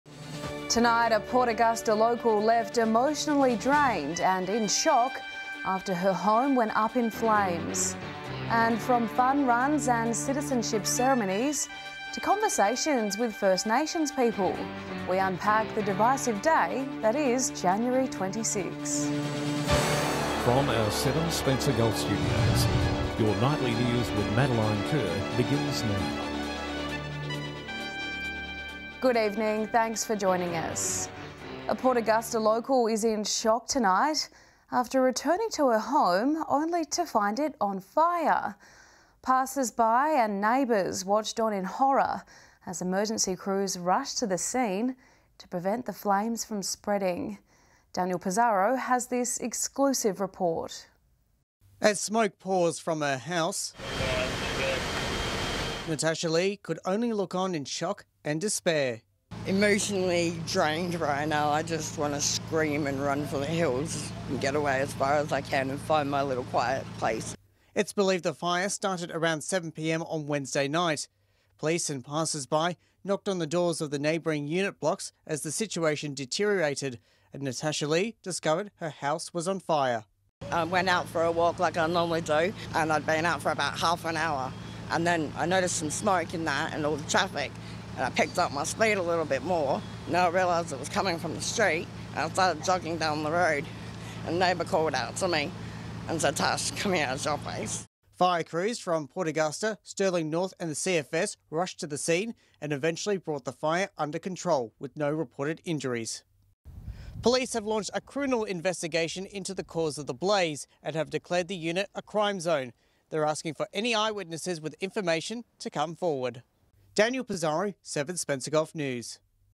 EXCLUSIVE: Suspicious Fire Leaves Occupant Heartbroken
Natasha-Lee Little is devastated after returning from a walk to see her home on Port Augusta's Frome Street up in smoke.

Police believe the fire was deliberately lit.